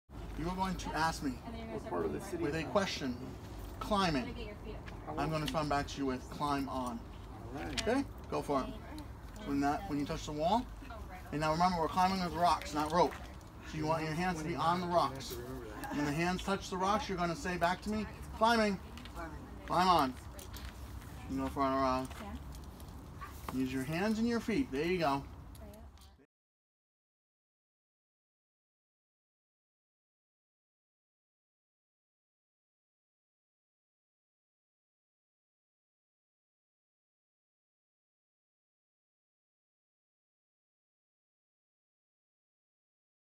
Climbing at Quincy Quarries
Students with Quincy’s recreation department got to try their hand at outdoor rock climbing in July at Quincy Quarries Reservation.